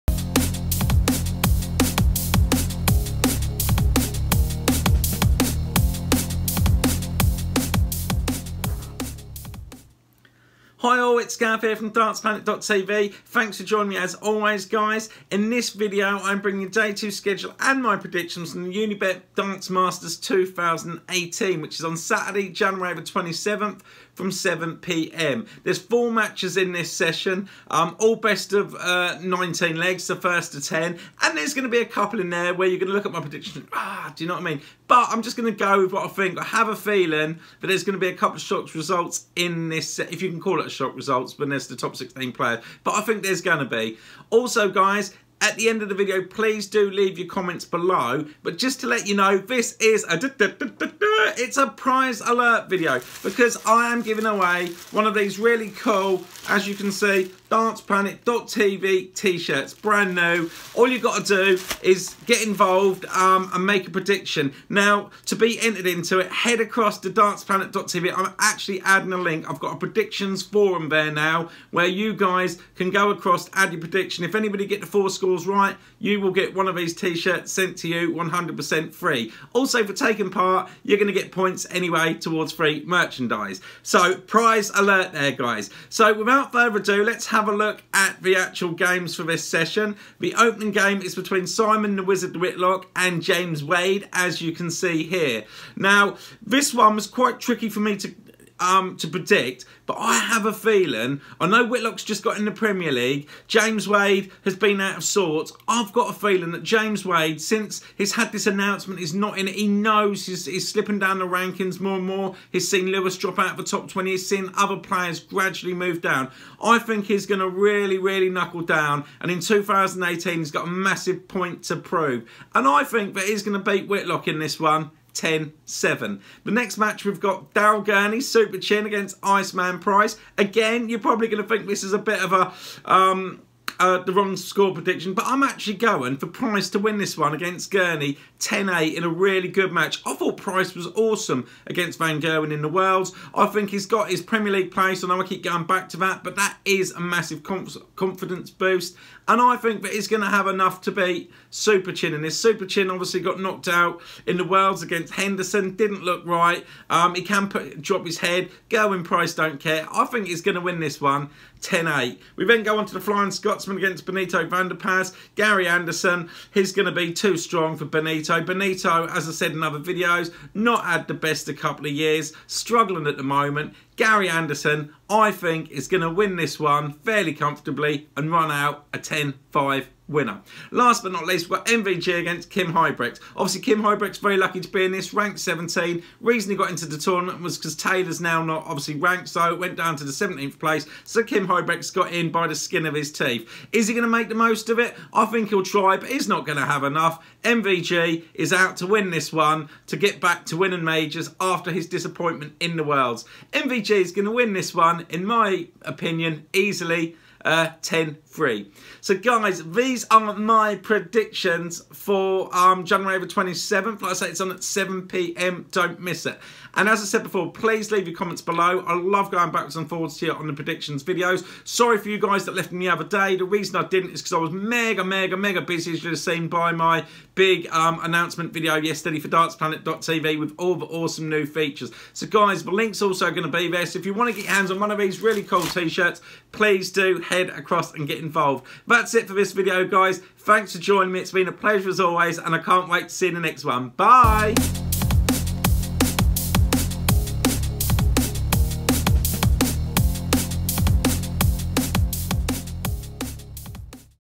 PRIZE ALERT | DAY 2 | UNIBET DARTS MASTERS 2018 | SCHEDULE & PREDICTIONS
This video features the schedule and my predictions for day 2 of the Unibet Darts Masters from the MK Arena Milton Keynes.

To ENTER THE PRIZE COMPETITION please visit this link

I have made the video fun by also adding my predictions as well as the schedule.

Saturday 27th January ( Evening session From 7pm )

My Predictions .

Simon Whitlock 7 - 10 James Wade
Daryl Gurney 8 - 10 Gerwyn Price
Gary Anderson 10 - 5 Benito van de Pas
Michael van Gerwen 10 - 3 Kim Huybrechts

What are your predictions?
What is your stand out match?
Do you think their could be a shock result?